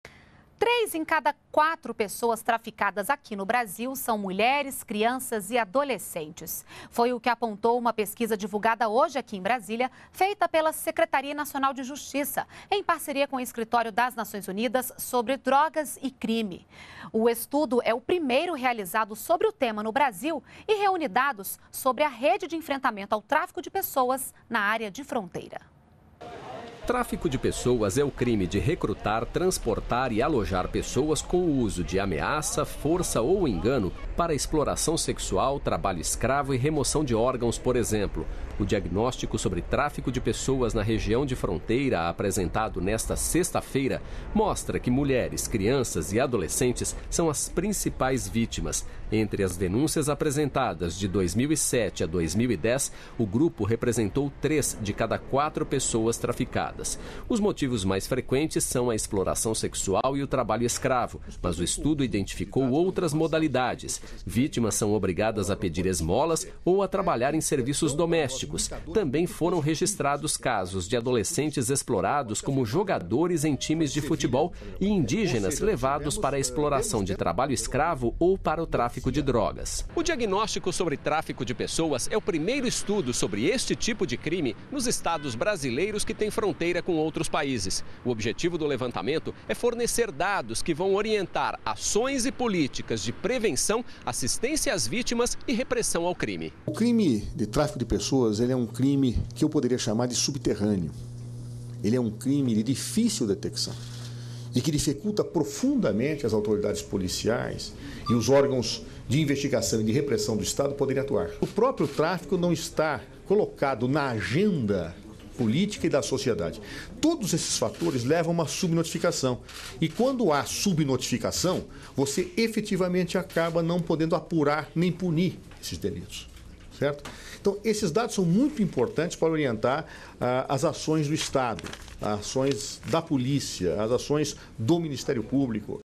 Exploração sexual e trabalho escravo são as principais finalidades do tráfico de pessoas no Brasil
NBR NOTÍCIAS - 18.10.13: Três em cada quatro pessoas traficadas no Brasil são mulheres, crianças e adolescentes. O dado integra a pesquisa "Diagnóstico sobre Tráfico de Pessoas nas Áreas de Fronteira no Brasil", divulgada nesta sexta-feira (19) pela Secretaria Nacional de Justiça, em parceria com o Escritório das Nações Unidas sobre Drogas e Crime. O estudo é o primeiro realizado sobre o tema no Brasil e reúne dados sobre a rede de enfrentamento ao tráfico de pessoas na área de fronteira. O documento mostra as modalidades mais comuns do crime, o modo de ação dos traficantes, as características da ação criminosa nos 11 Estados de fronteira e define encaminhamentos concretos para que sejam desenvolvidas políticas públicas na área de atendimento às vítimas e repressão aos criminosos. Entre 2005 a 2011, pelo menos 475 pessoas foram vítimas do tráfico de pessoas. A pesquisa destaca a incidência de tráfico de pessoas para fins de trabalho escravo no Rio Grande do Sul, Paraná, Pará, Amazonas, Mato Grosso e Mato Grosso do Sul. Para fins de exploração sexual, há uma concentração no Amapá, Pará, Roraima, Mato Grosso e Mato Grosso do Sul. Também há casos de exploração da mendicância e da servidão doméstica de crianças e adolescentes.